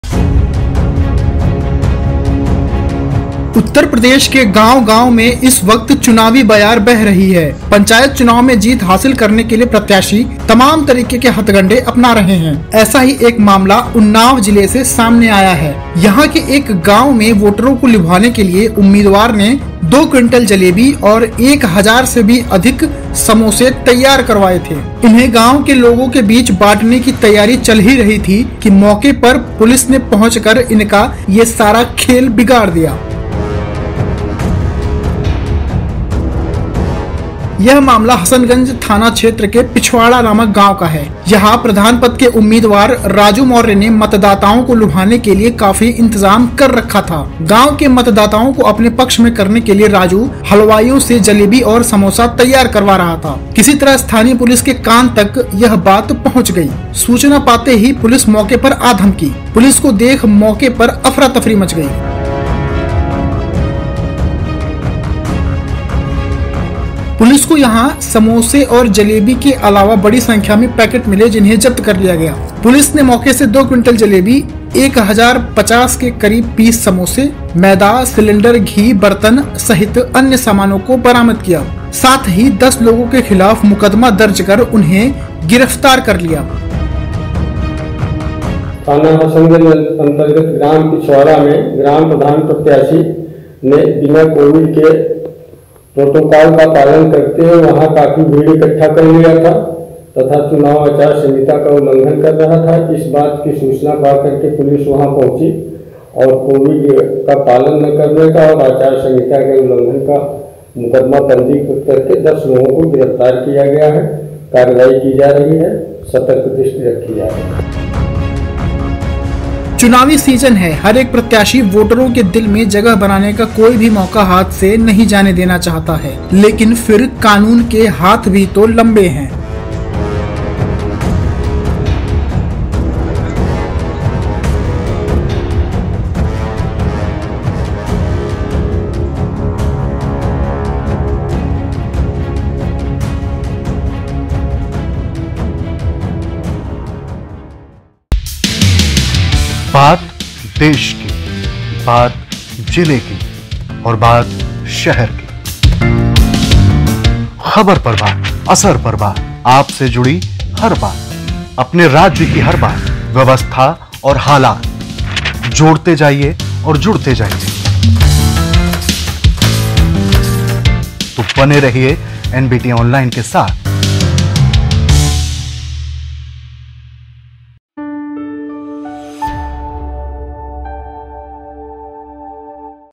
UP Panchayat Election: वोटर्स को लुभाने के लिए हजारों समोसे, 2 क्विंटल जलेबी, लेकिन पहुंच गई पुलिस
उत्तर प्रदेश पंचायत चुनावों में जीत हासिल करने के लिए प्रत्‍याशी तमाम तरह के हथकंडे अपना रहे हैं। उन्‍नाव के एक गांव में वोटरों को लुभाने के लिए उम्‍मीदवार ने 2 क्विंटल जलेबी और 1000 से ज्‍यादा समोसे तैयार करवाए। इन्‍हें गांव के लोगों के बीच बांटने की तैयारी चल ही रही थी कि मौके पर पुलिस ने पहुंचकर सारा 'खेल' बिगाड़ दिया। पुलिस ने इस मामले में 10 लोगों को अरेस्‍ट किया है। साथ ही, जलेबी-समोसे को अपने कब्‍जे में कर लिया।